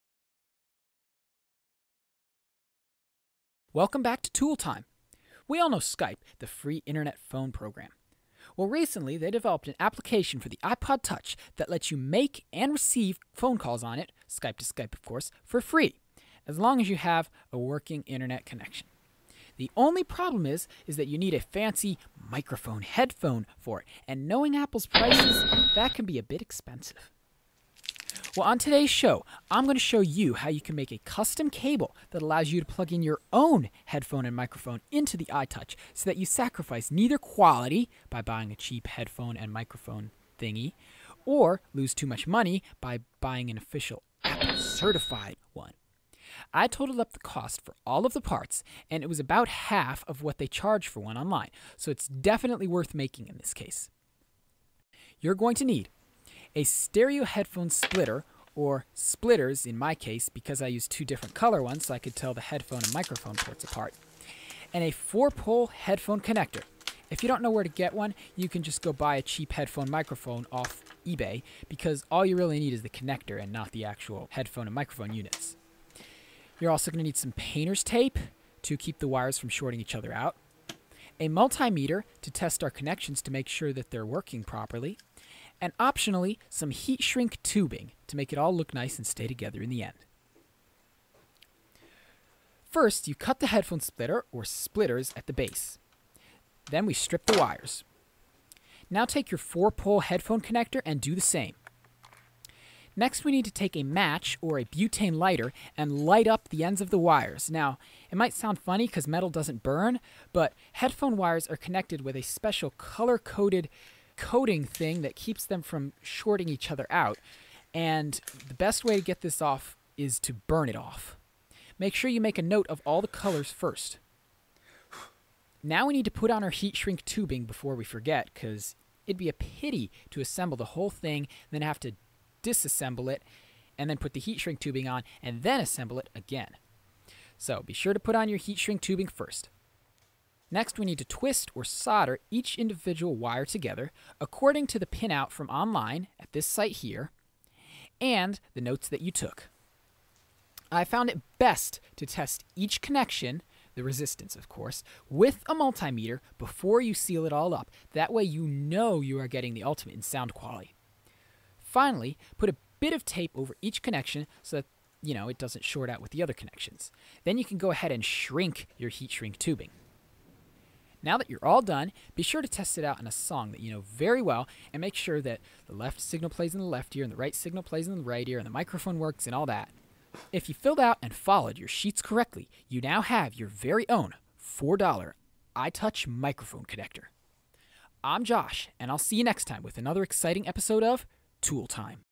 Tool Time #2: Mic Port for the iTouch
On this episode of Tool Time, Josh walks through how to make a cable that allows you to plug in a microphone and headphone into your iPod Touch so that you can use the skype application. You loose neither quality by using a junky apple wannabe, nor loose too much money by buying a $30 official apple version.

The total cost of this project is around $4, which is much less then what you can buy a pre-made version for online.